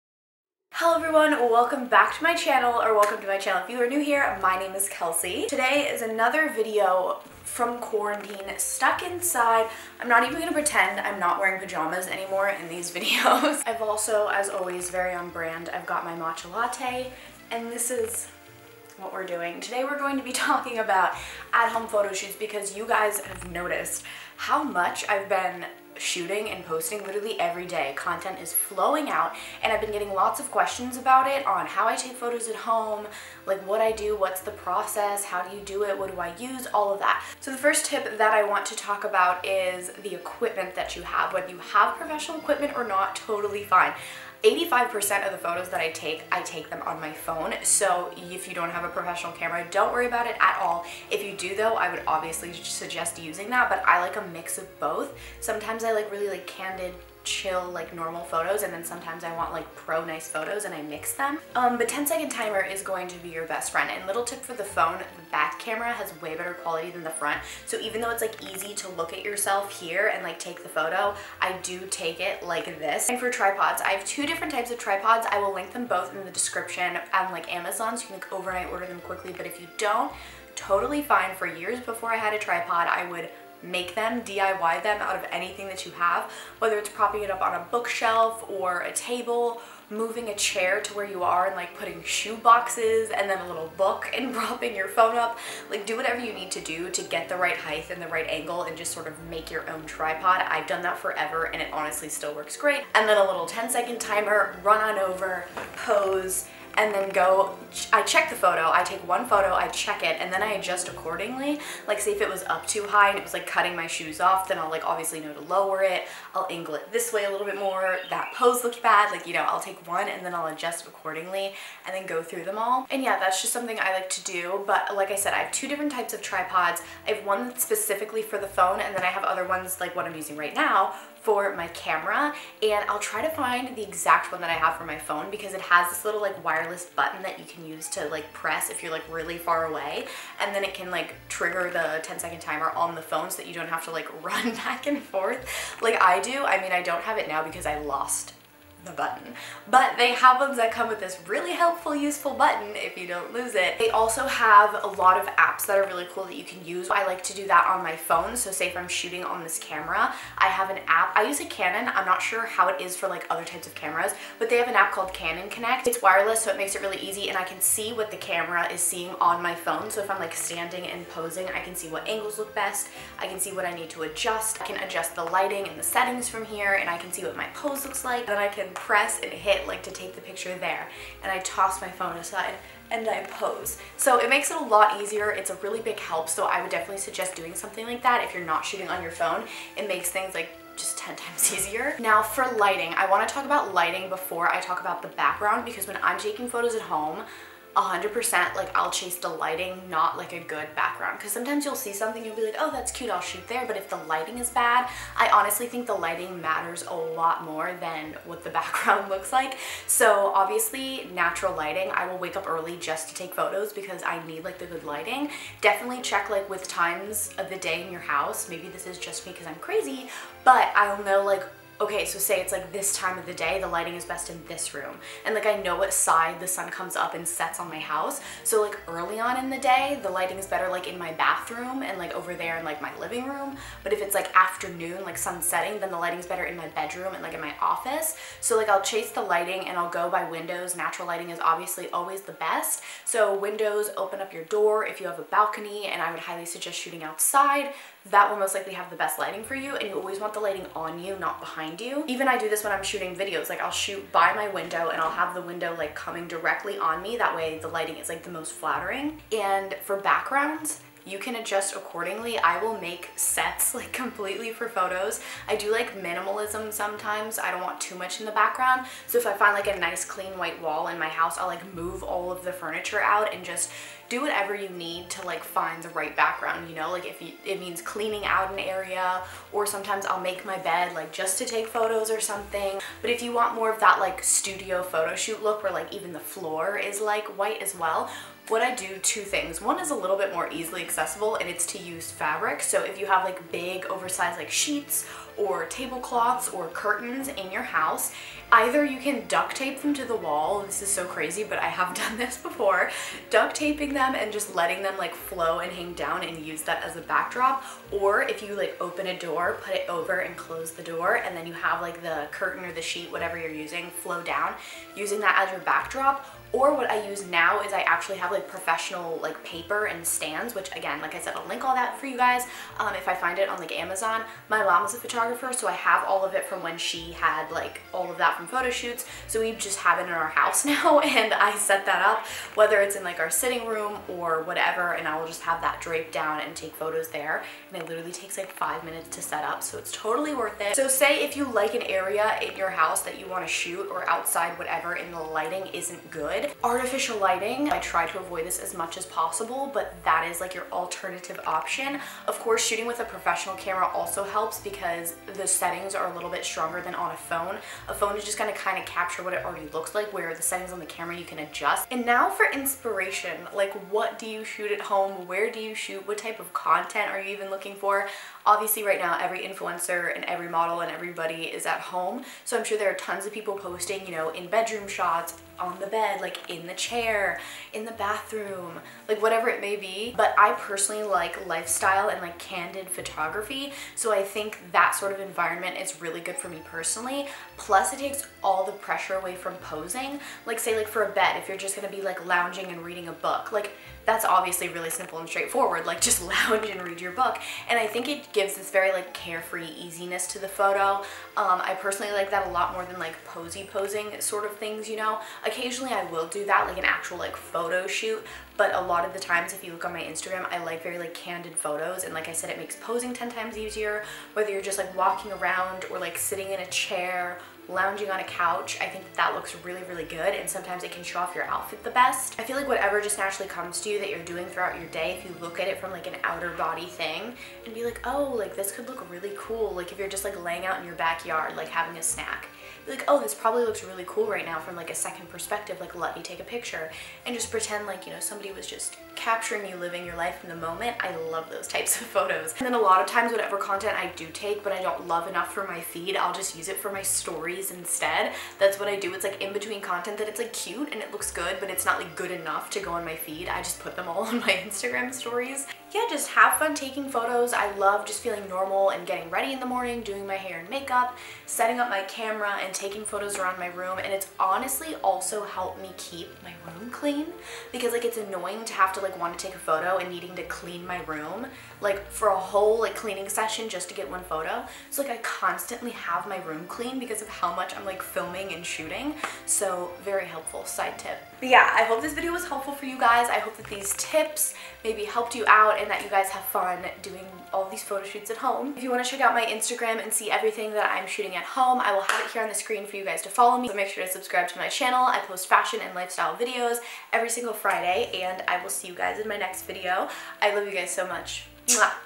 HOW TO: At-Home Photoshoot! How I take my Instagram Pictures
Here are some of my tips & essentials on how I take all my photos at home! I hope this was helpful for you since I had so many questions! See you in my next video Xx - Kelsey

| 1 Corinthians 10:13 |

My Links:
We Heart It: rosiecheeksss

Tripod for Iphone
Background paper

(Use name Kelsey Simone or Kelsey Besanson)